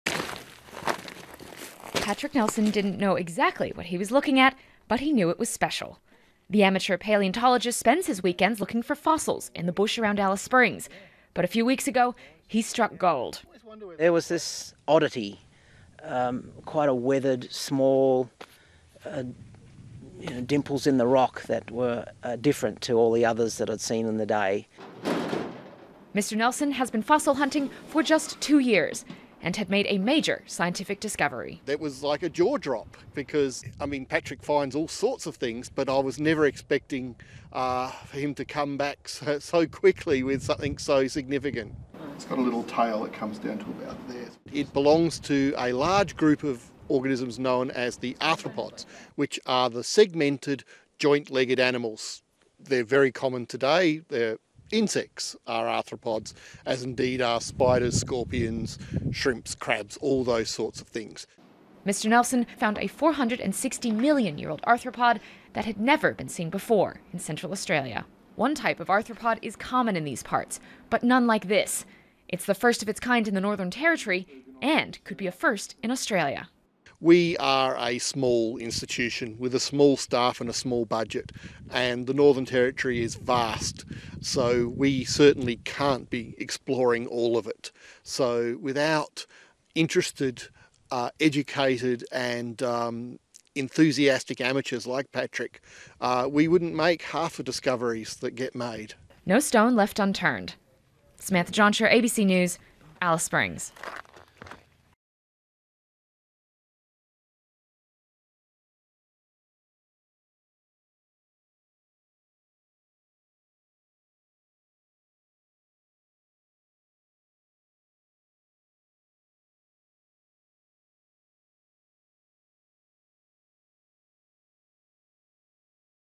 Citizen scientist makes major 460-million-year-old fossil find in the Northern Territory | ABC News
It's not everyday that you go for a weekend walk and make a major scientific discovery, but for one citizen scientist, that's just what happened.

One Alice Springs man has struck gold and found a never-before-seen 460 million-year-old fossil.

Samantha Jonscher reporting.